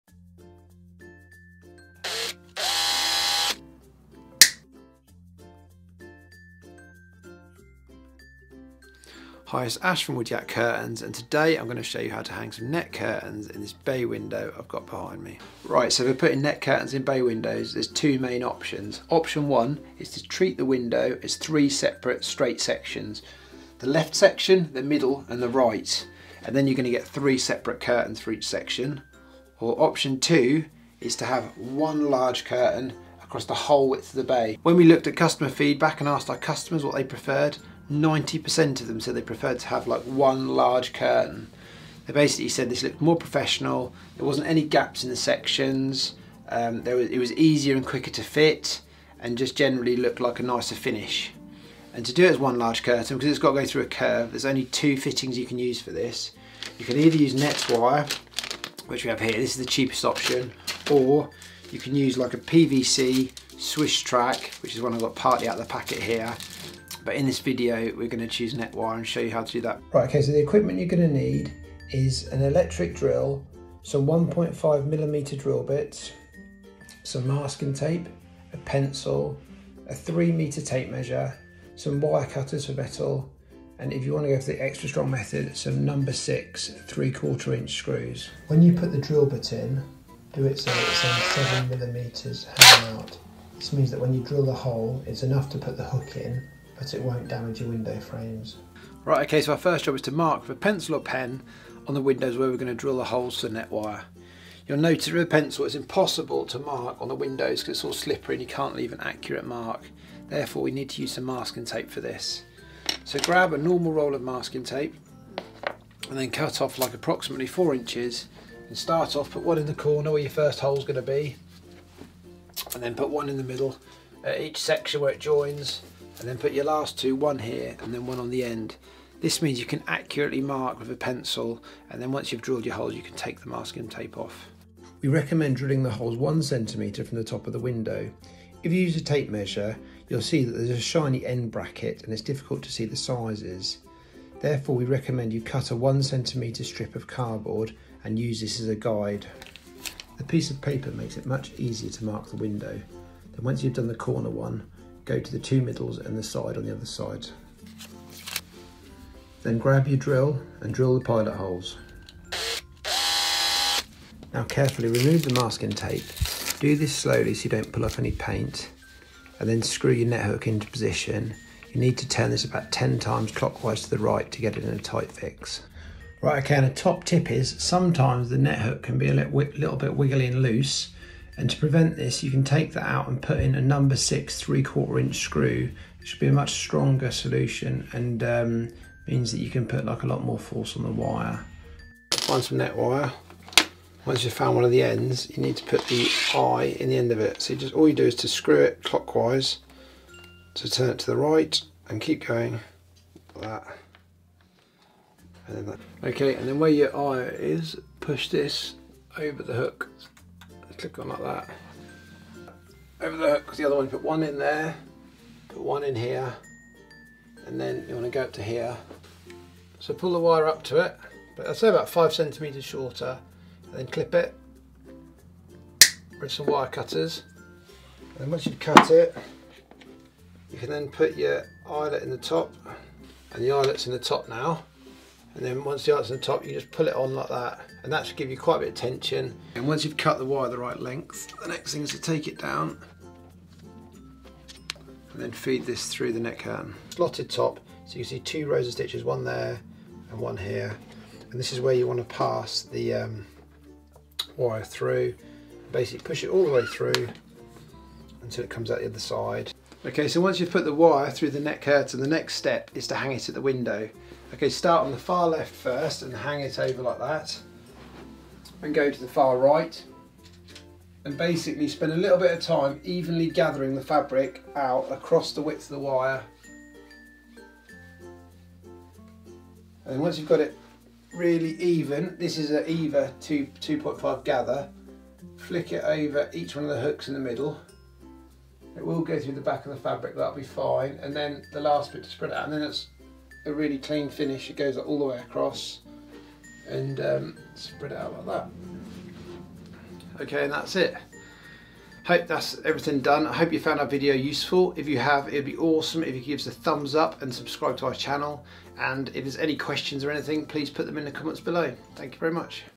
How to Hang Net Curtains in Bay Windows
This video Shows how to hang Net Curtains in Bay Windows.

This can be done with either net wire or a curtain track but in this video we are going to show how this can be done with net curtain wire.

For this you will need some equipment.

You will need an electric drill some 1.5mm drill bits. Some masking tape, a tape measure, some wire cutters and some No 6 (3/4) inch screws

To view our full range of net curtains and voiles and other window dressings please visit our